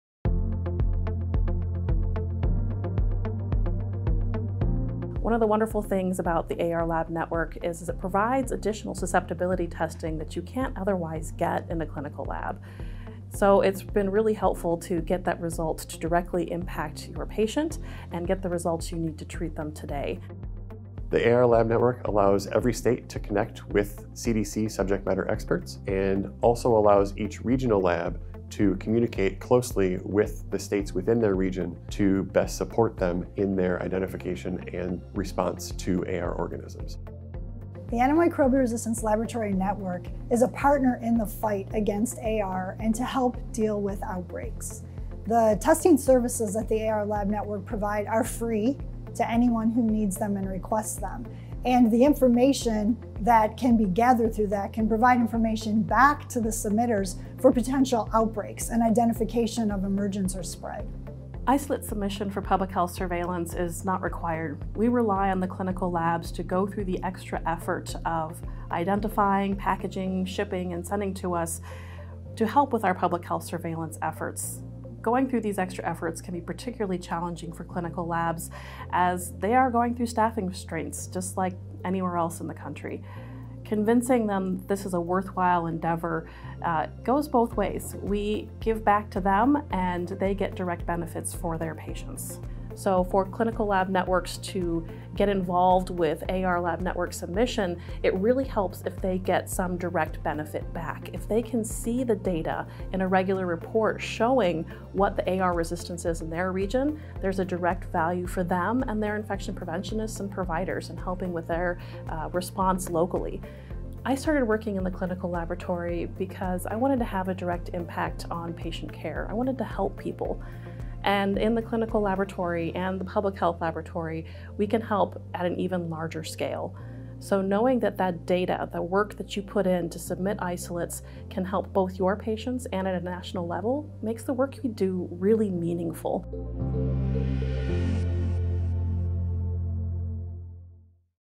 Improve Testing Locally
CDC’s Antimicrobial Resistance Laboratory Network (AR Lab Network) offers testing services that can supplement the capabilities at local laboratories. The testing services that the AR Lab Network provide are free to anyone who needs them and the information that can be gathered through that can provide information back to the submitters for potential outbreaks and identification of emergence or spread.